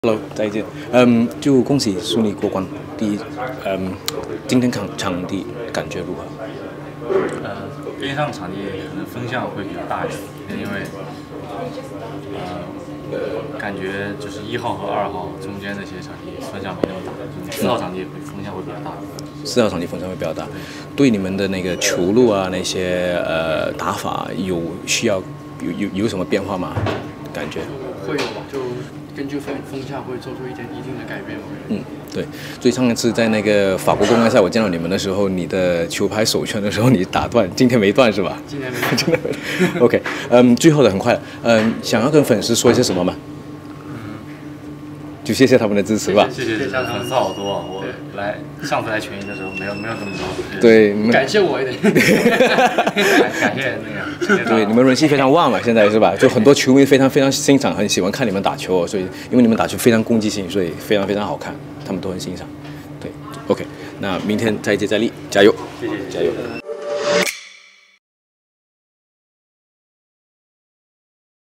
Liang Wei Keng Wang Chang (China) All England 2023 Interview
I interview Liang Wei Keng and Wang Chang of China after their first round match at the 2023 Yonex All England Badminton Championships.